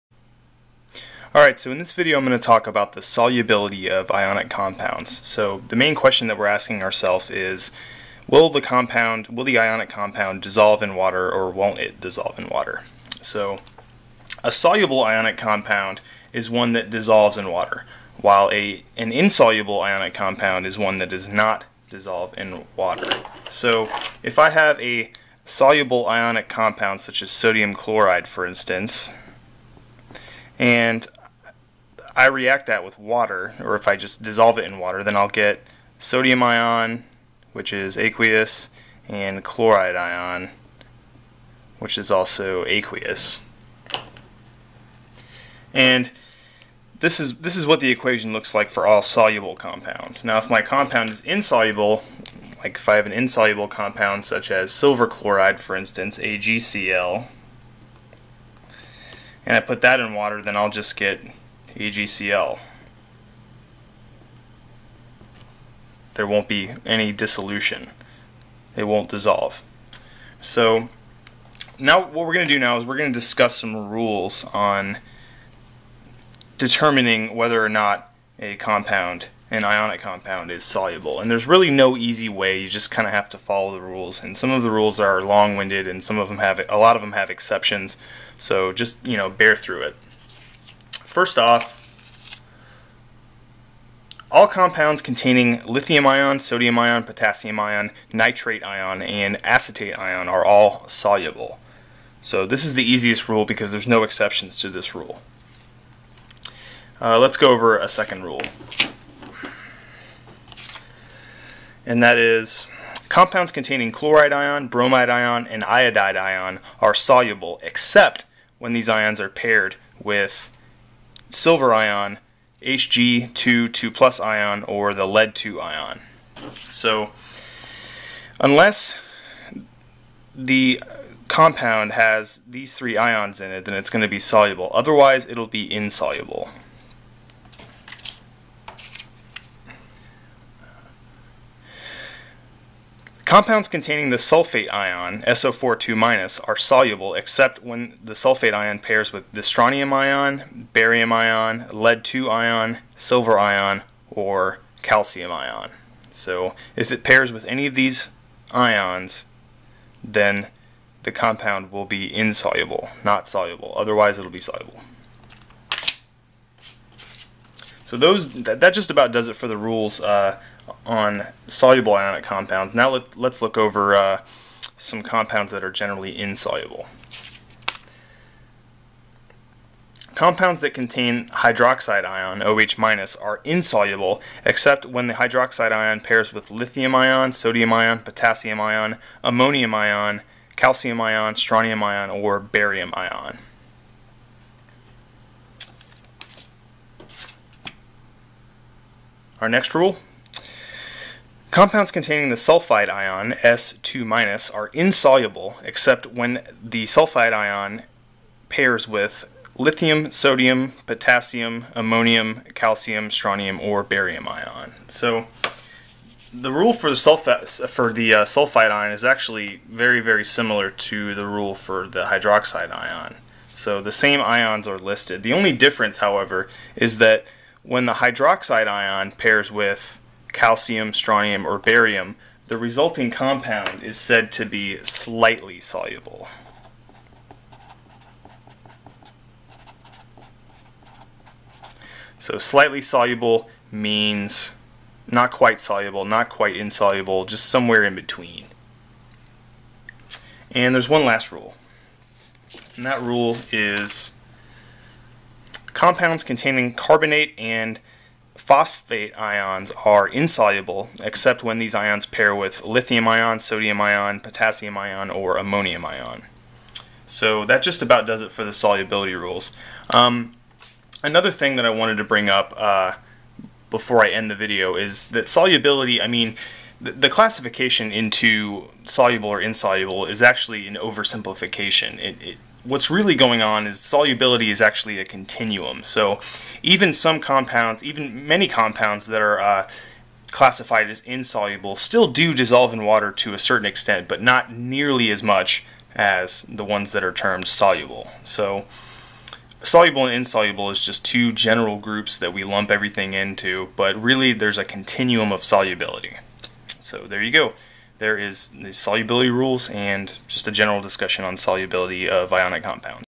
Solubility of Ionic Compounds: Basics and Rules
This video is a basic discussion on the solubility of ionic compounds and the rules that are used to determine whether an ionic compound is soluble or insoluble.